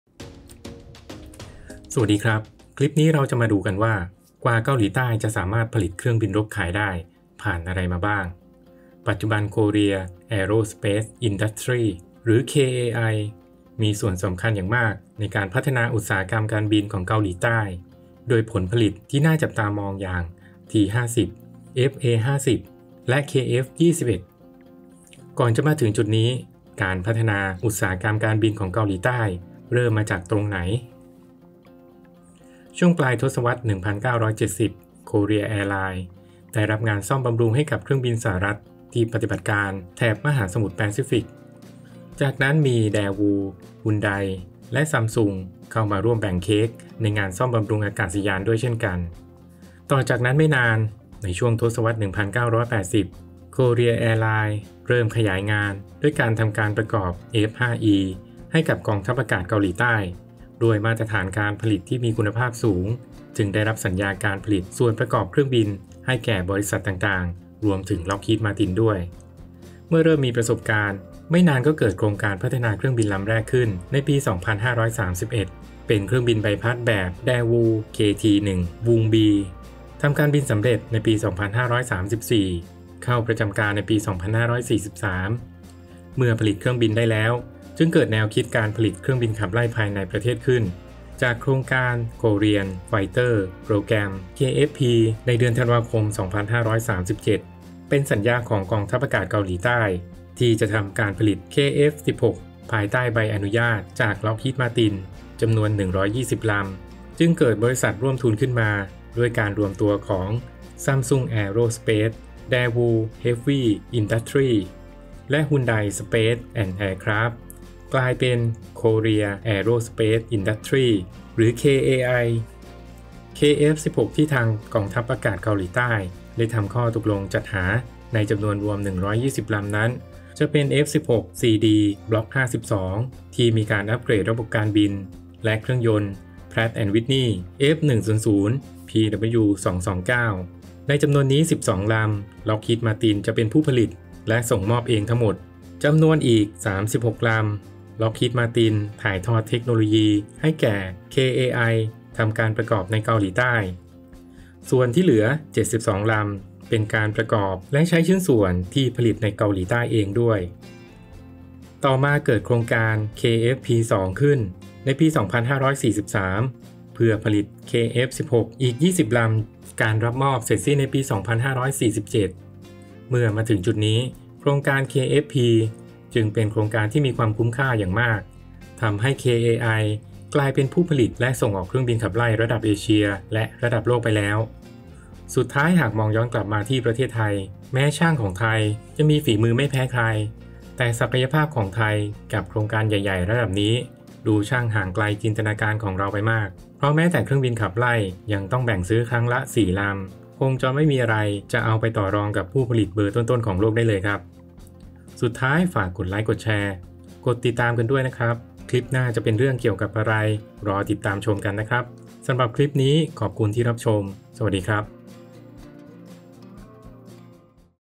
เกาหลีให้กลายเป็นผู้ผลิตเครื่องบินรบได้อย่างไร
ความเป็นมาของอุตสาหกรรมการบินของเกาหลีใต้